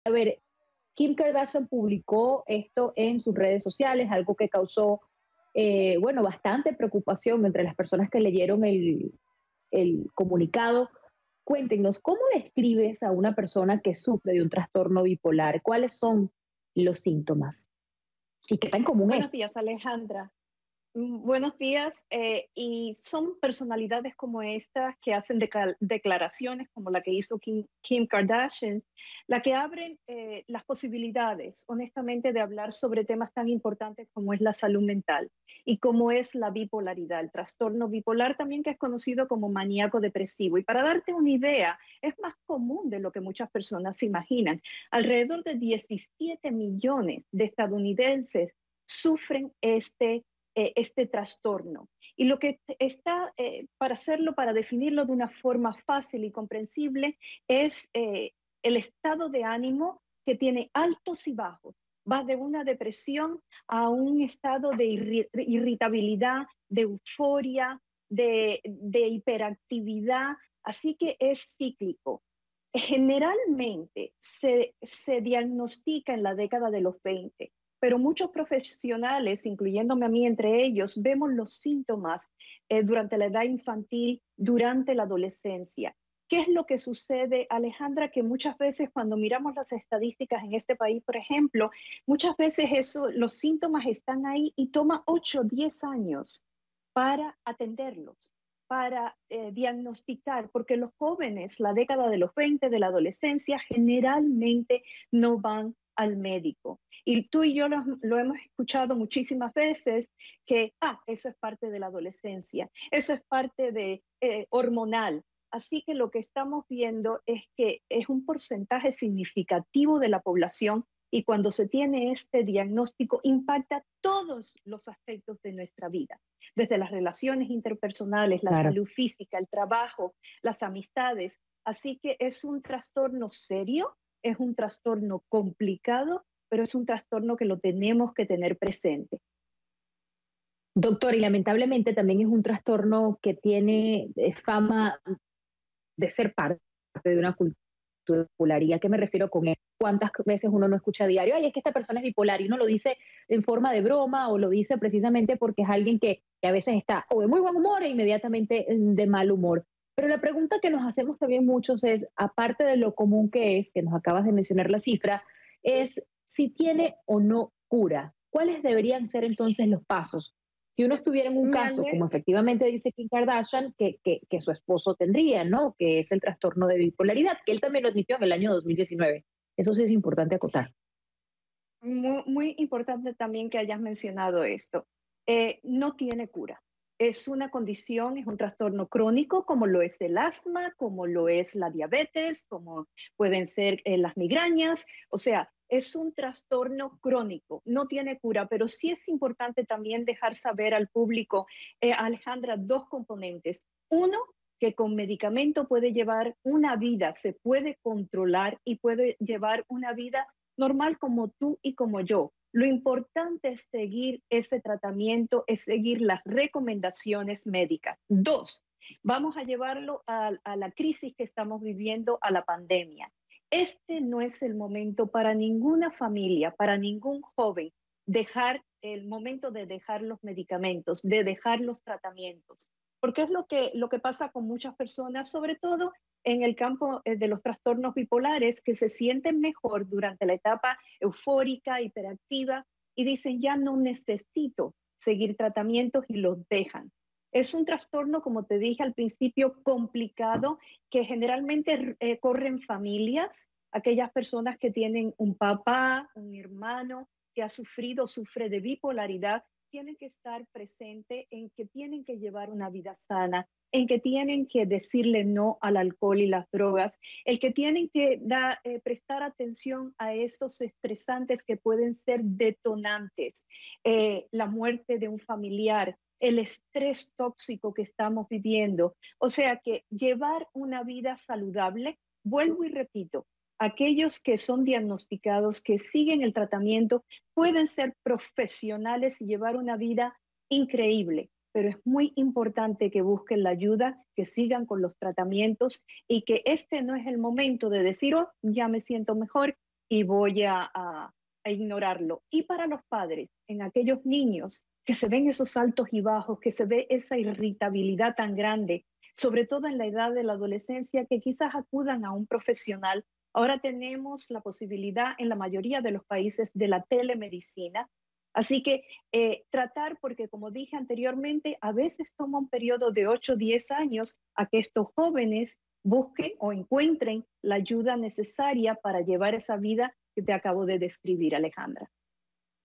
Esto es lo que puede experimentar una persona con trastorno bipolar
A pesar del tabú, el trastorno de bipolar es más común de lo que se cree. Varias celebridades han confesado padecerlo. En uno de los casos más recientes, Kim Kardashian habló sobre la salud mental de su esposo Kanye West, de quien afirma haber sido diagnosticado con trastorno de bipolar.

Describió su situación como “increíblemente complicada y dolorosa”. ¿En qué consiste esta afección mental? La Dra. Marisa Azaret explica cuáles son los episodios que viven las personas que lo padecen y a qué edad puede empezar a detectarse los síntomas.